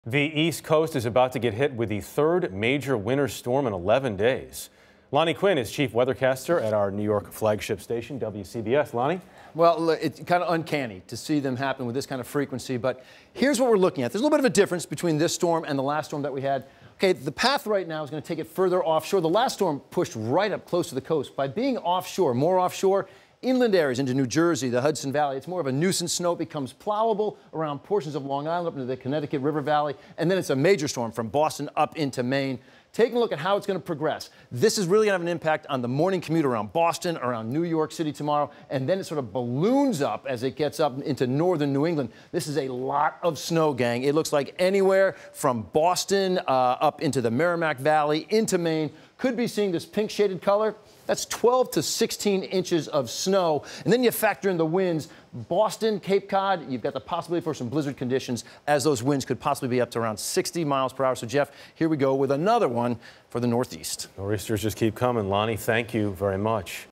New England braces for a 3rd nor'easter in 10 days
A third nor'easter is set to hit the East Coast, and could bring more than a foot of snow to parts of New England. CBS New York's chief weathercaster Lonnie Quinn has the forecast.